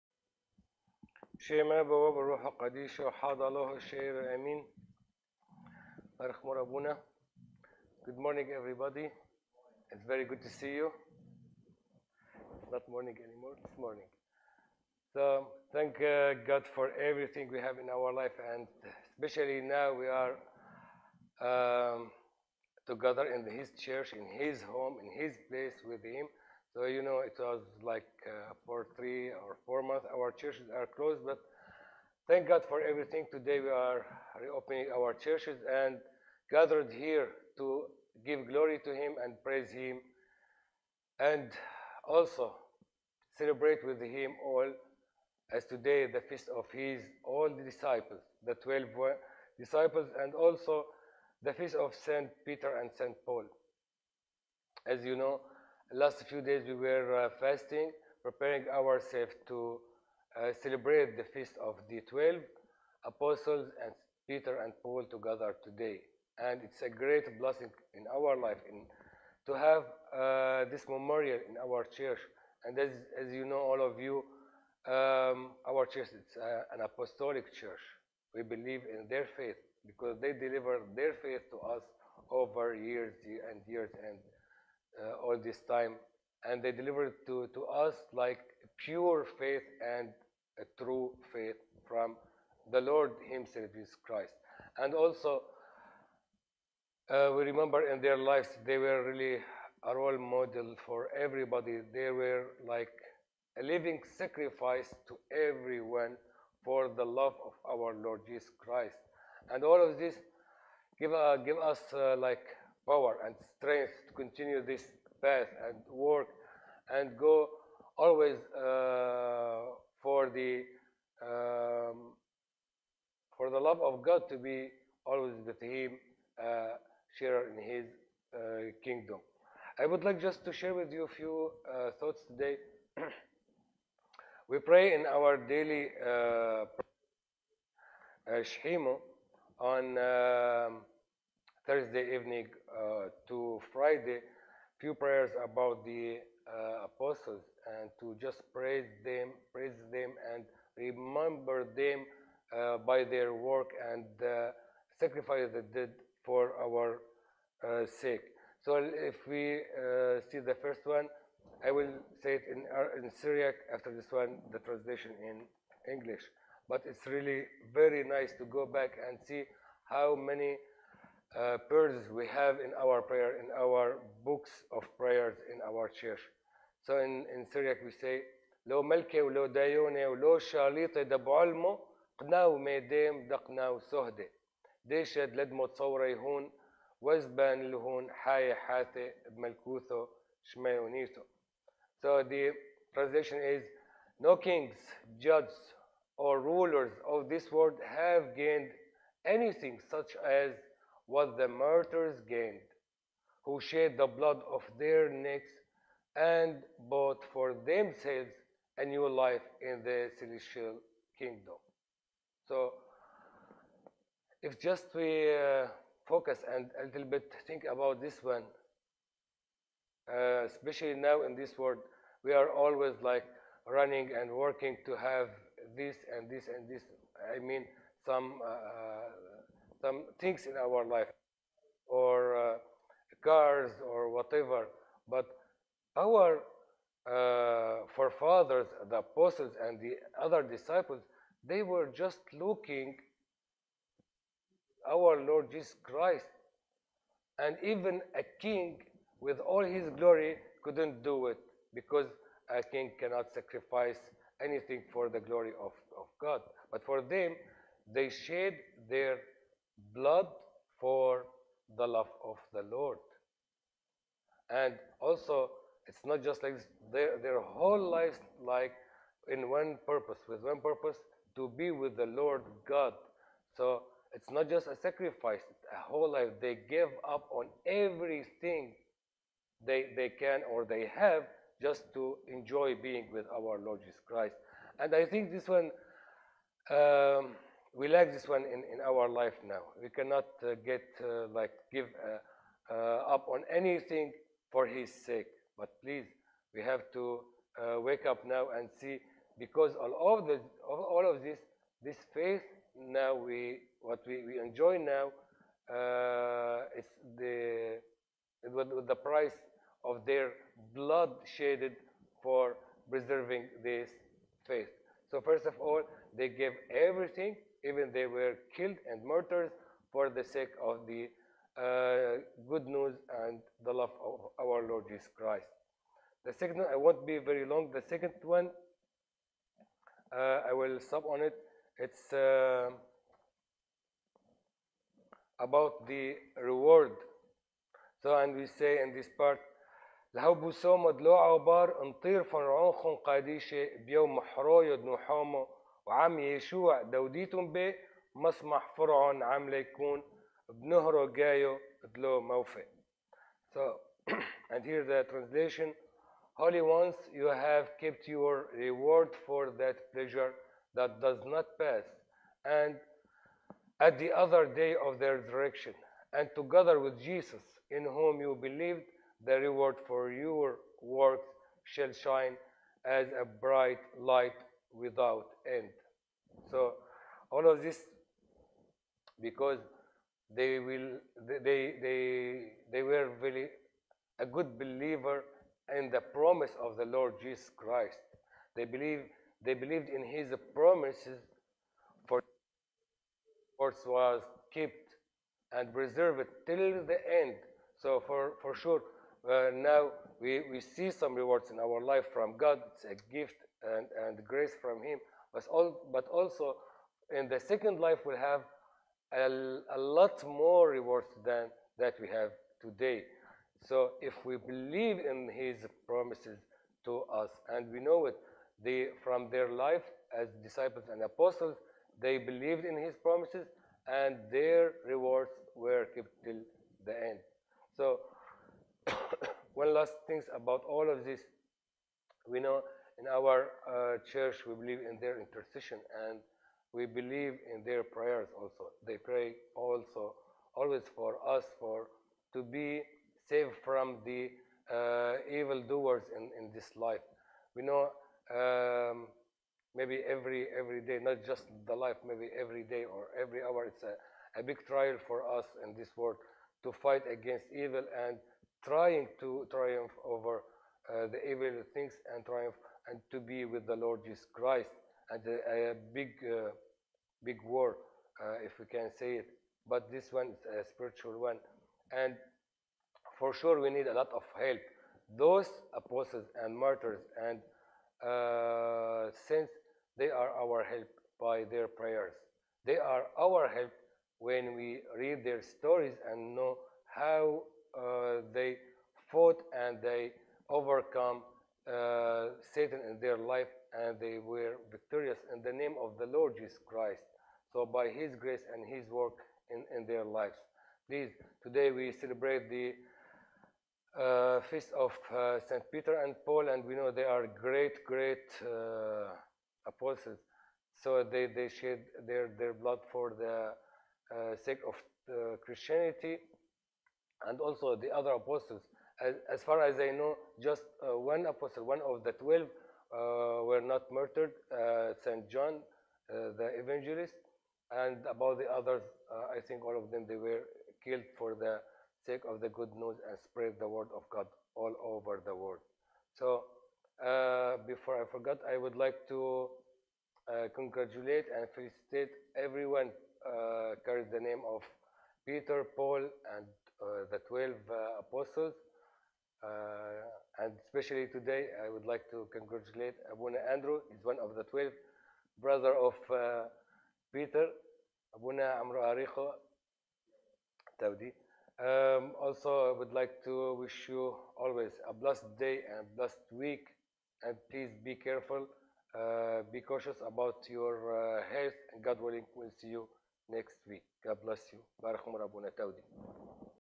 Feast of the Disciples, Peter & Paul - June 28, 2020 - Assyrian Orthodox Church of the Virgin Mary
This past Sunday we celebrated the feast of the 12 disciples, and the feast of St. Peter and St. Paul. As Father Saliba notes, it is a great blessing in our life to have this memorial in our church, an apostolic church. We believe in their faith, and they delivered to us the pure faith from our Lord and Savior Jesus Christ. They were a living sacrifice for everyone in our world to Jesus Christ.

They shed their blood for the love of the Lord. Their whole life was with one purpose, to be with the Lord God. We lack this in our life. We have to wake up now and see what we enjoy now comes with the price of the blood they shed for our faith, the good news of our Lord Jesus Christ.